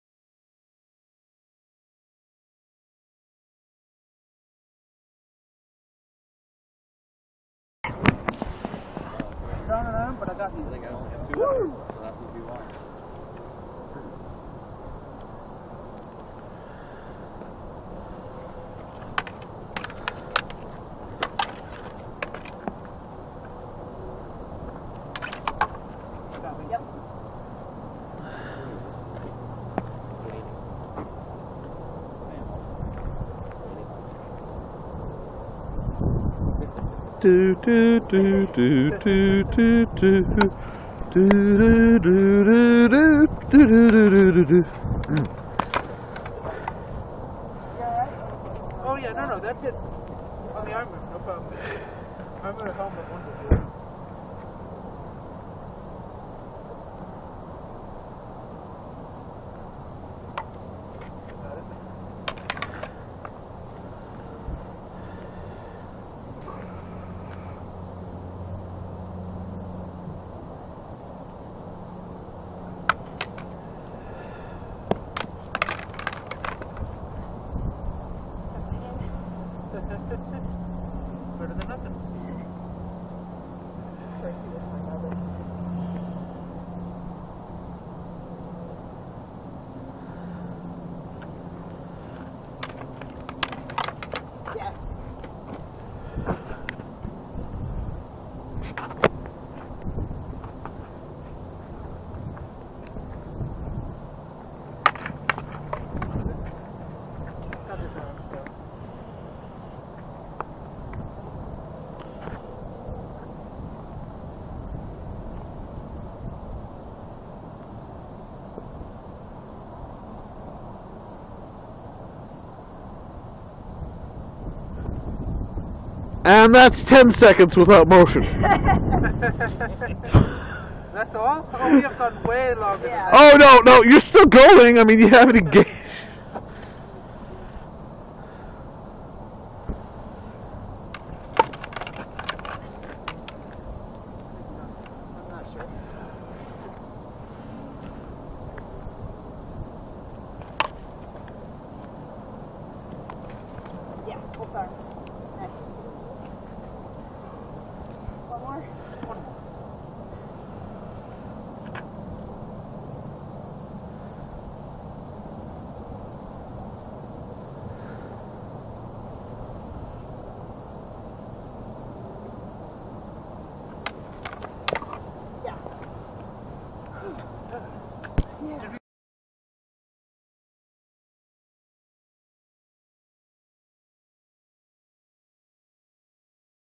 Trilaina vs Jim: Twin Dussacks vs Sabre and Buckler 2-13-15
Sparring match between Trilaina and Jim. At 2/13/15